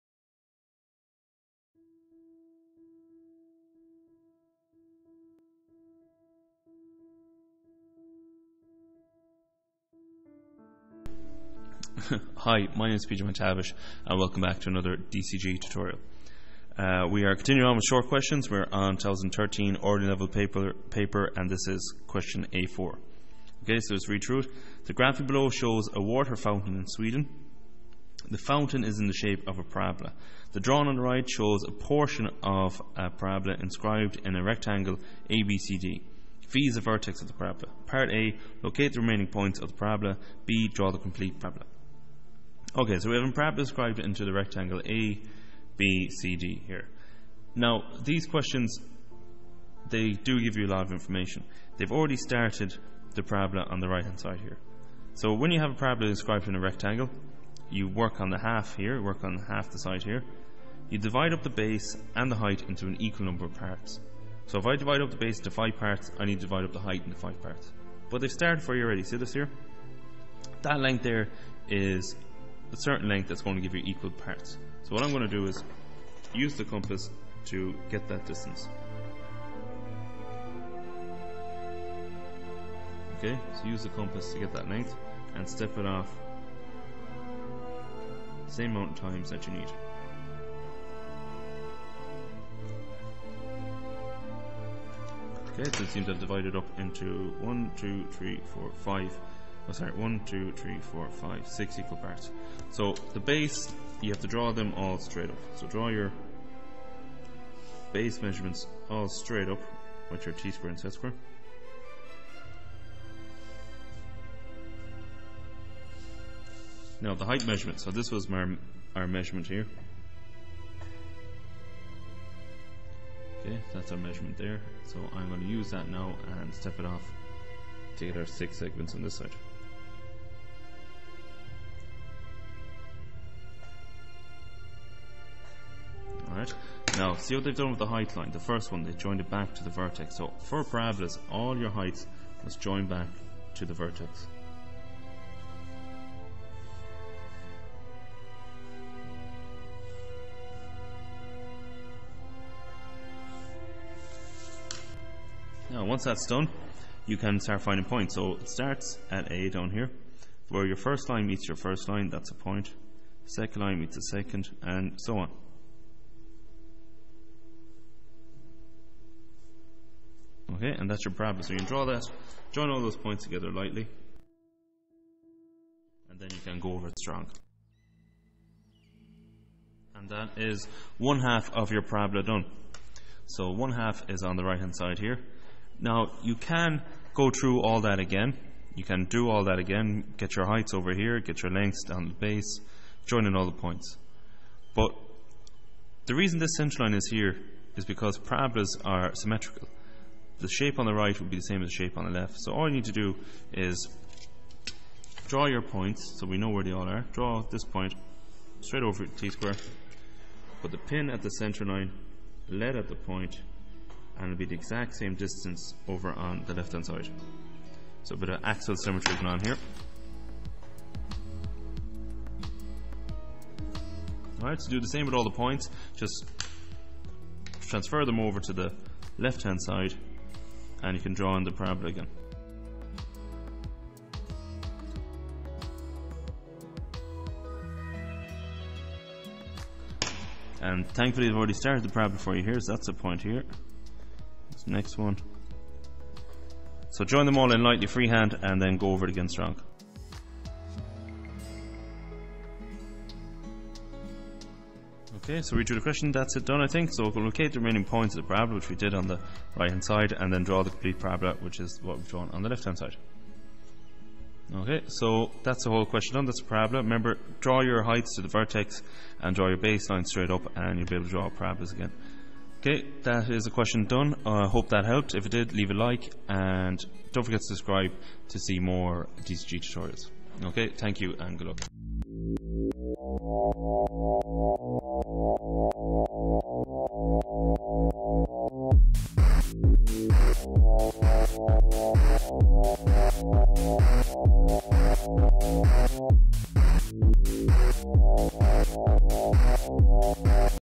DCG: 2013 Ordinary Level Question A-4
Conic Sections short question from the 2013 paper.

This question is based on a Parabola.